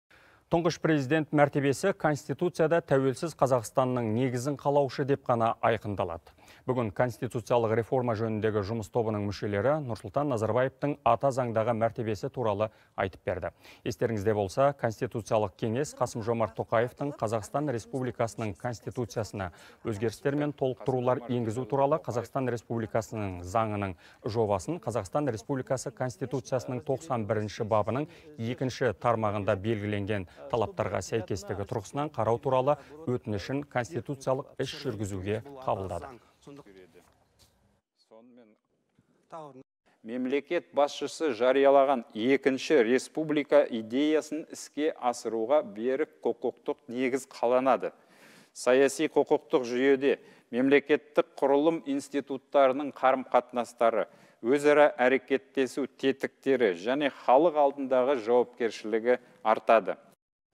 Тұңғыш Президент мәртебесі Конституцияда «Тәуелсіз Қазақстанның негізін қалаушы» деп айқындалады
Конституциялық кеңестің аппарат жетекшісі Нұрсұлтан Назарбаевтың Ата заңдағы мәртебесі туралы айтып берді.

«Осылайша,  Конституцияға Президенттен басқа лауазымды тұлғаны кіргізудің қажеті жоқ», – деді Бақыт Нұрмұханов. Естеріңізде болса, Конституциялық Кеңес Қасым-Жомарт Тоқаевтың «Қазақстан Республикасының Конституциясына өзгерістер мен толықтырулар енгізу туралы» Қазақстан Республикасы Заңының жобасын Қазақстан Республикасы Конституциясының 91-бабының 2-тармағында белгіленген талаптарға сәйкестігі тұрғысынан қарау туралы өтінішін конституциялық іс жүргізуге қабылдады.